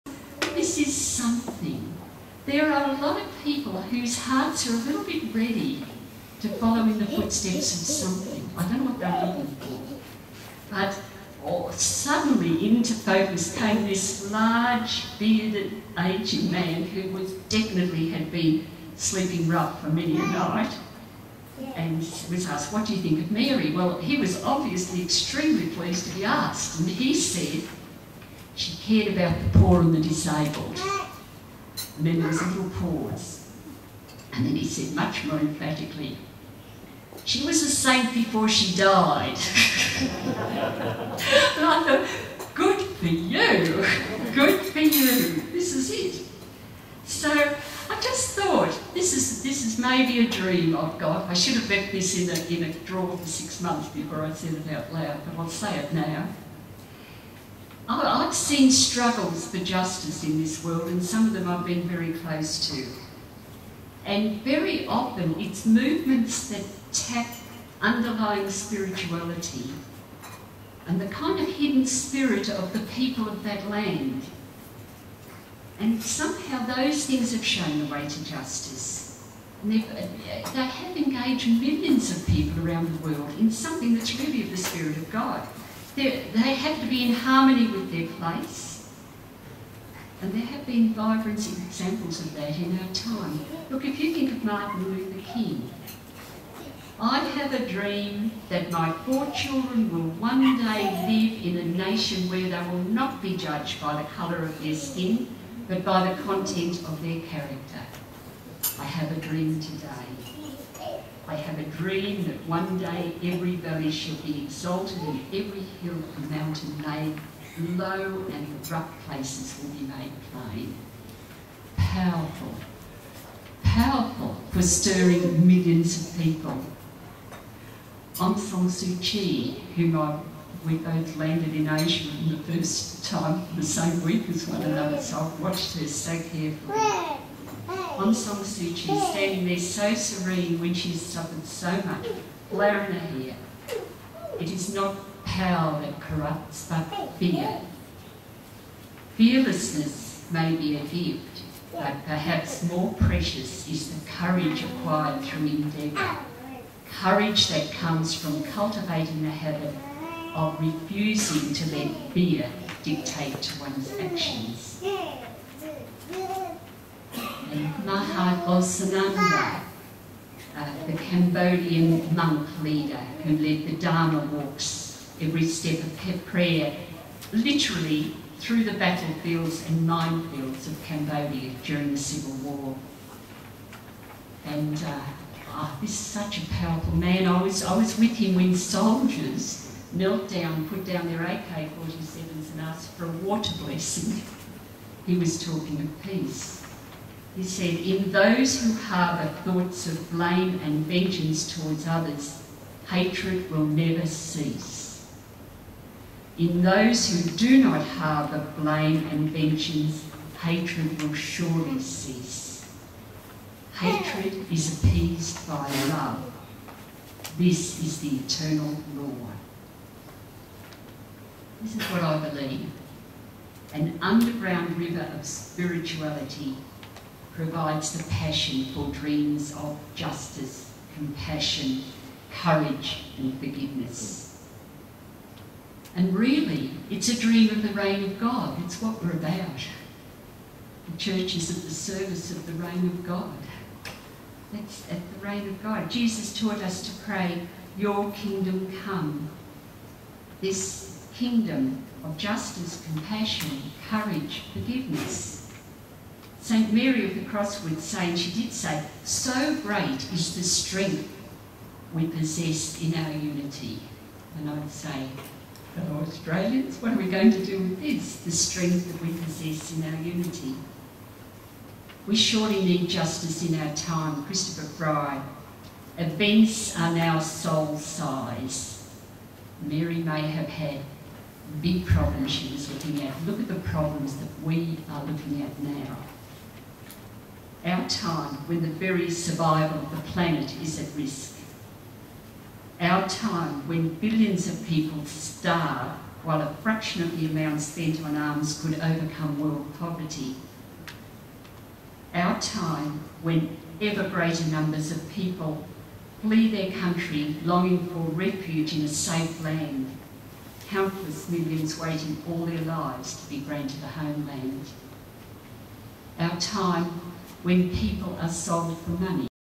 2010 Rerum Novarum lecture by Justice & Peace Office of the Archdiocese of Melbourne (Part 4 )
Each year the Office for Justice and Peace of the Catholic Archdiocese of Melbourne conducts the Rerum Novarum Lecture to commemorated the great 1891 Encyclical of Pope Leo XII on the Rights of Workers. The Encyclical was the first encyclical of the Church in the modern era to discuss social issues and formed the basis for the Church Social Teaching. This year the Lecture was delivered by Joan Healy a sister of the Congregation of St Joseph of the Scared Heart (The Josephites), the order of nuns established by Mary MacKillop. This was a particular tribute to the canonisation of Australia's first saint, St Mary of the Cross MacKillop.

Office for Justice and Peace of the Catholic Archdiocese of Melbourne